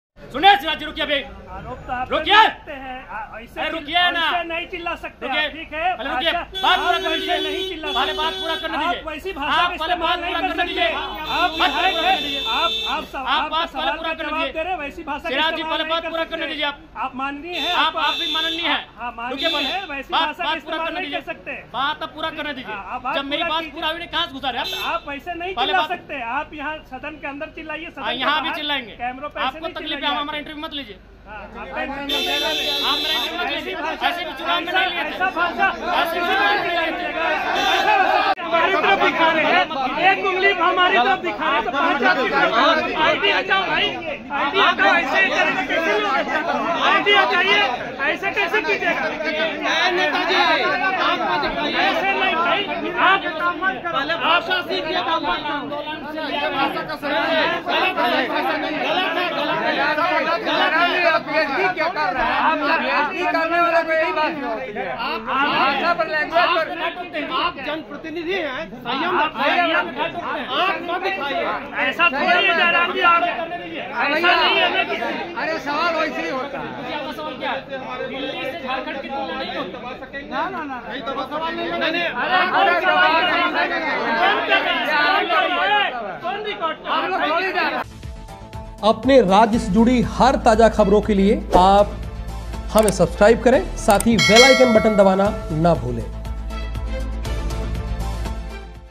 पत्रकार पर भड़के जयराम महतो... कहा- मत लिजिए मेरा इंटरव्यू
पत्रकार पर भड़के जयराम महतो...
कहा- मत लिजिए मेरा इंटरव्यू